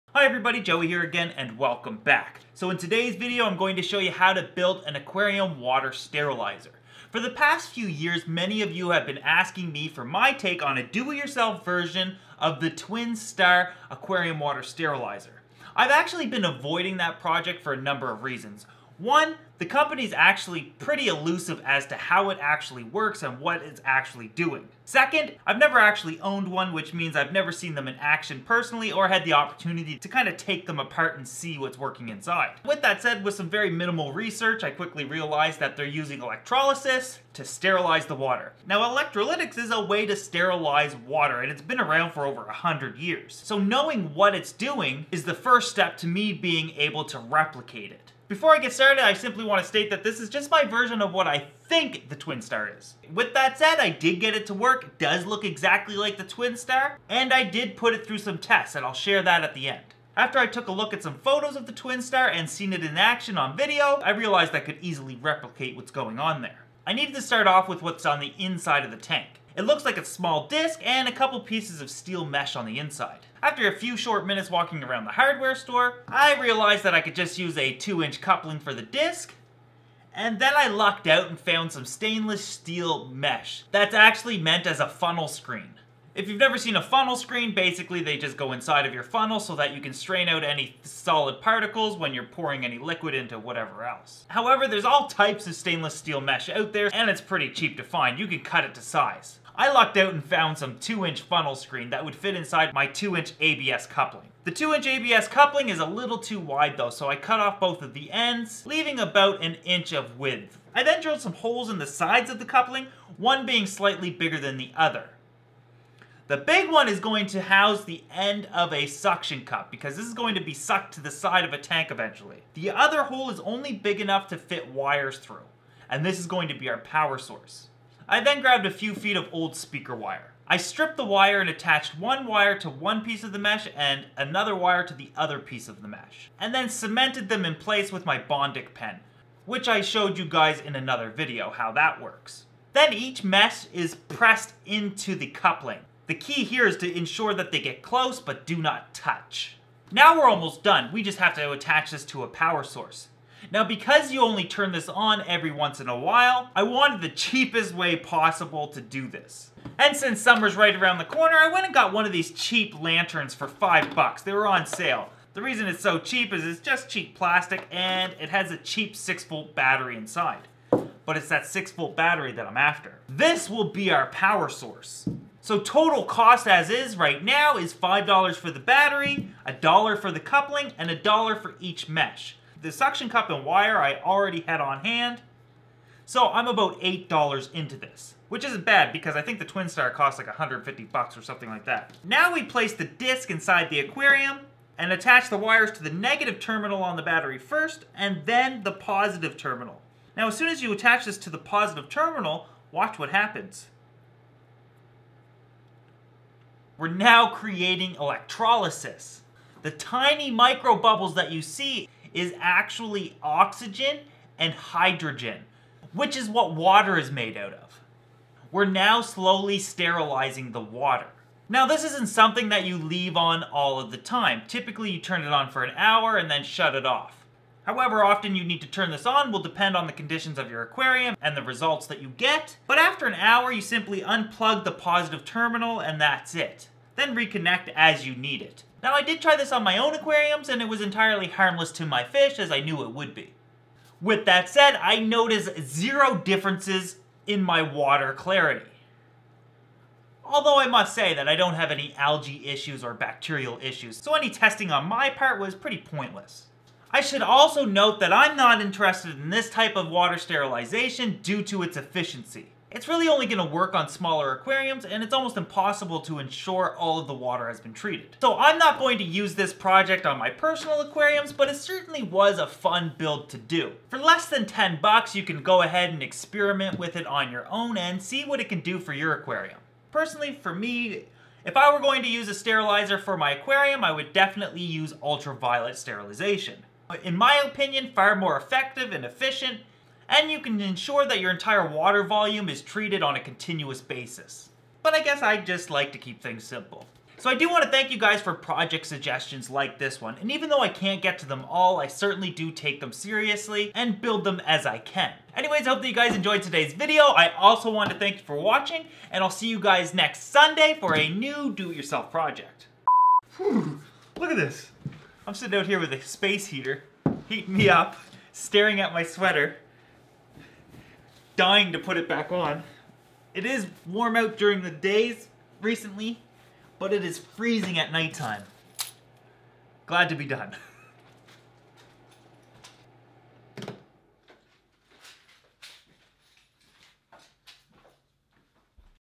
HOW TO: $8 Aquarium sterilizer
In today's video, i show you how to build an aquarium water sterilizer that will only cost you $8.